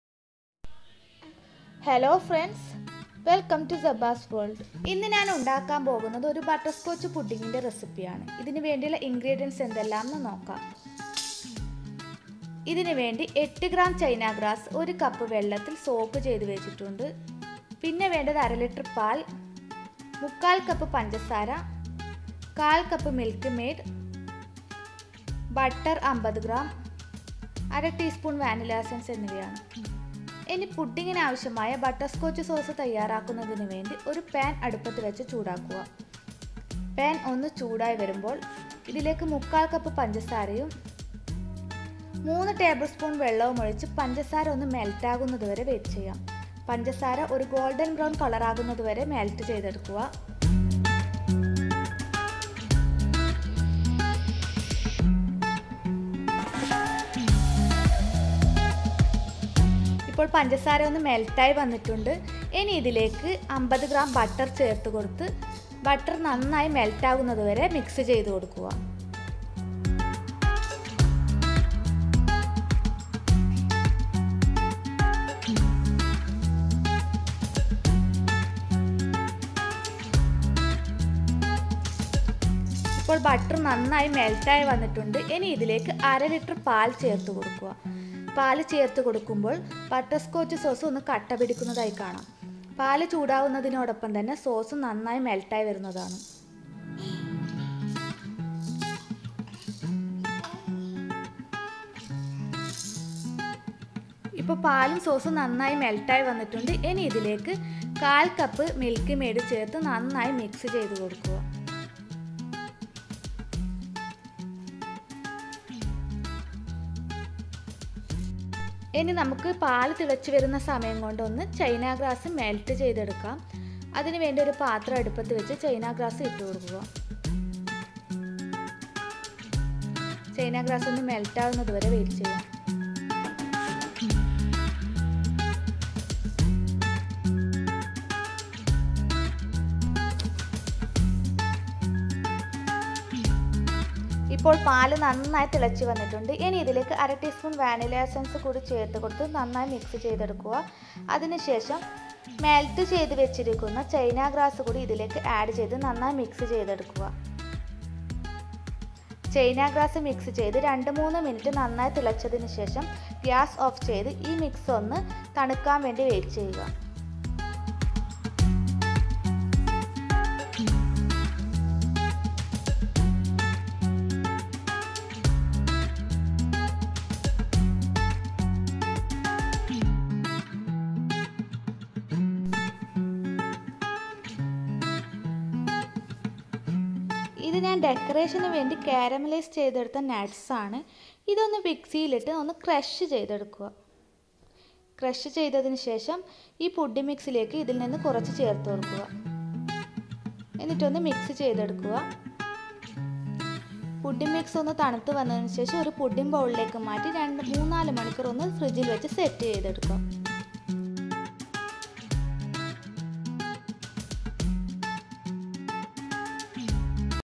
ബട്ടർ സ്കോച്ച് പുഡ്ഡിംഗ്|Easy Butter Scotch Pudding receipe in malayalm||Zebas World
Ingredients:
China grass-8gm
Milk-500ml
Sugar-3/4 cup
Milk maid-1/4 cup
Butter-50gm
Vanilla extract-1/2tsp
Cashew nuts-2tbsp
Water-1cup